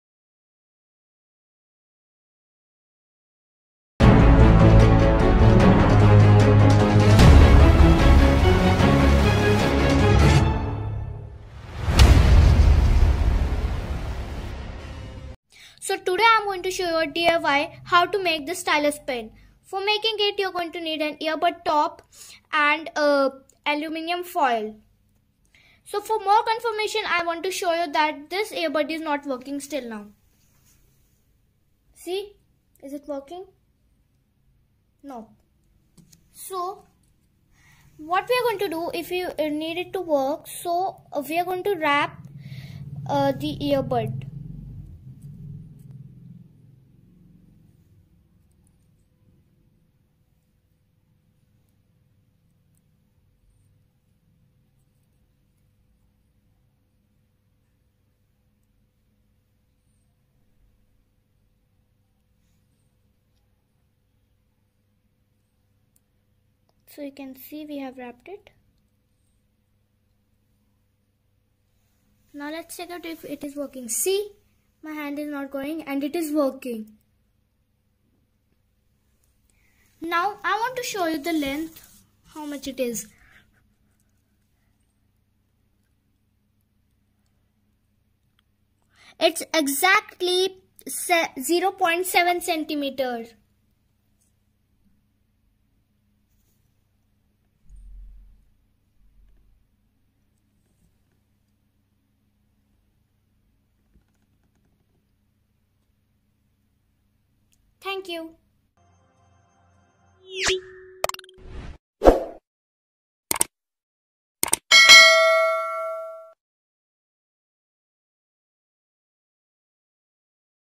Smallest stylus pen
The record for making the smallest stylus pen was set by Rishika Kamboj (born on March 31, 2010) of Panipat, Haryana. She made the smallest stylus pen (measuring 0.8 cm) using ear bud and aluminium foil, as confirmed on February 11, 2022.